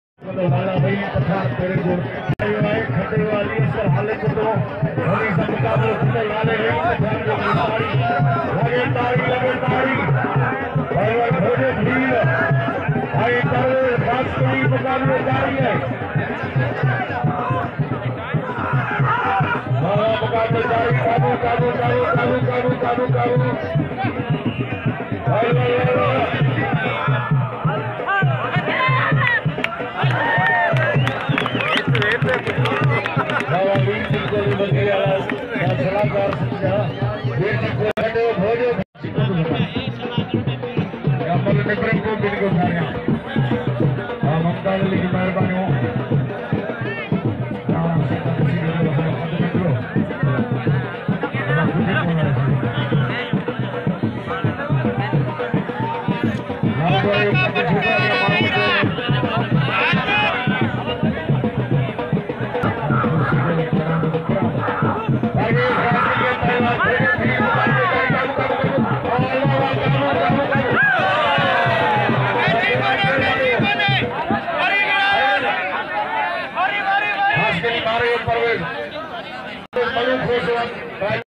Pehlwan Parvez Khaskile vs Bhojo Bheel Malakhro Sarkari Mithi Video number 105
Pehlwan Parveez Khaskile
Bhojo Bheel
Malakhro Sarkari Mithi Tharparker